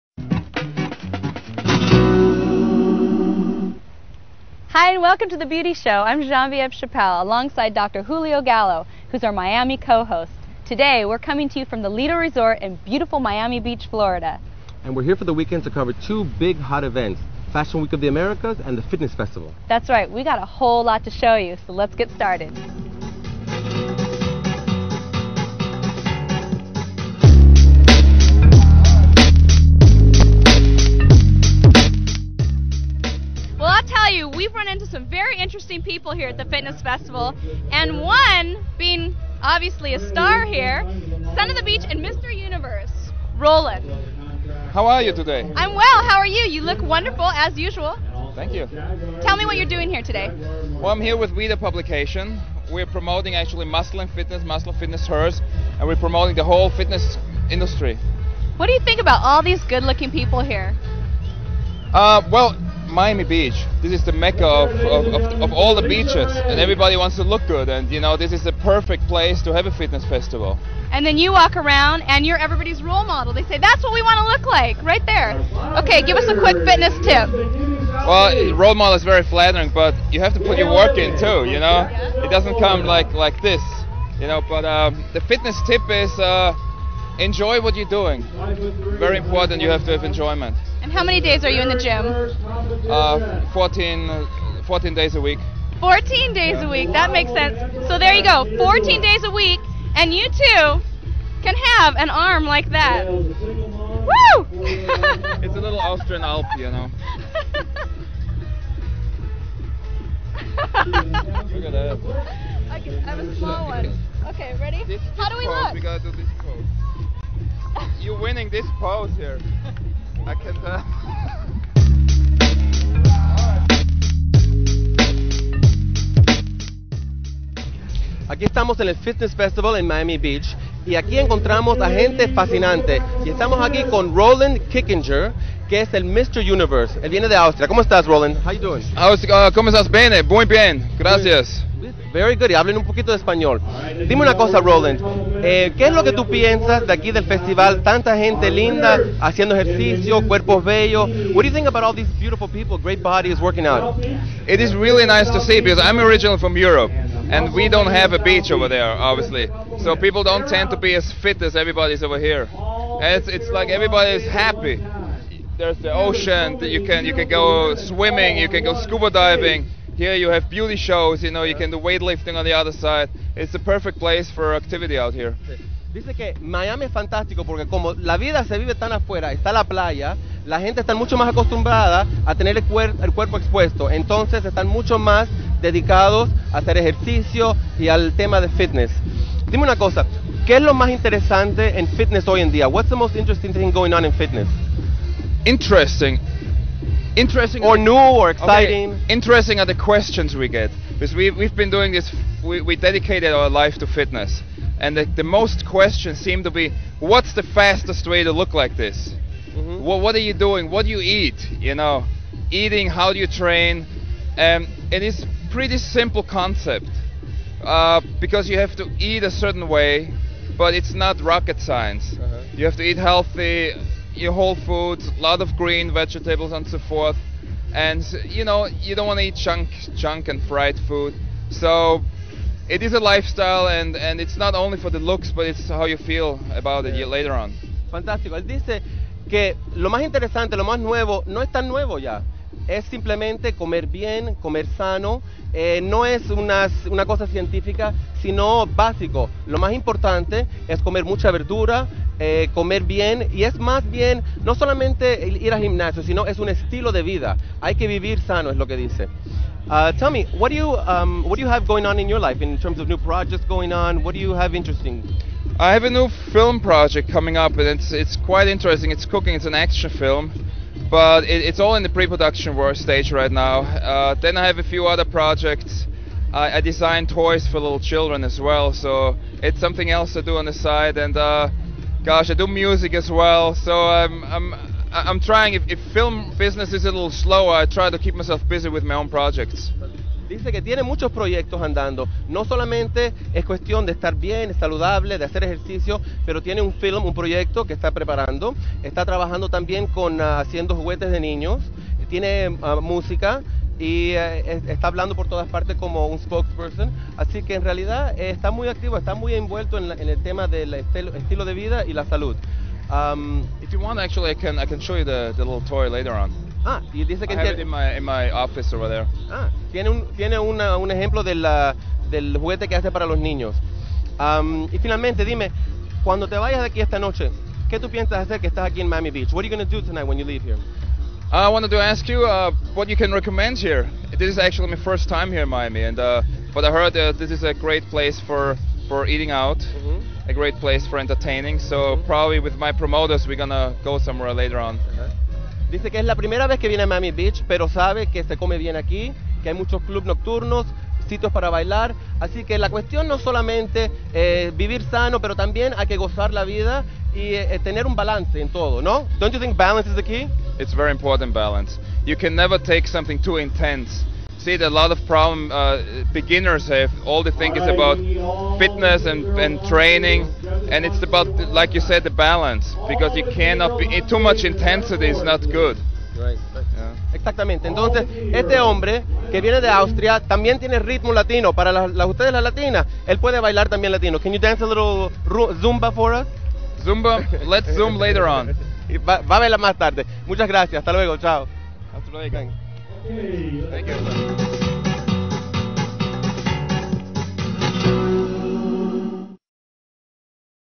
Miami Fashion & Fitness Week - En Espanol
Dr. Julio Gallo joins correspondent host Genevieve Chappell as a guest host during Miami Fashion and Fitness Week.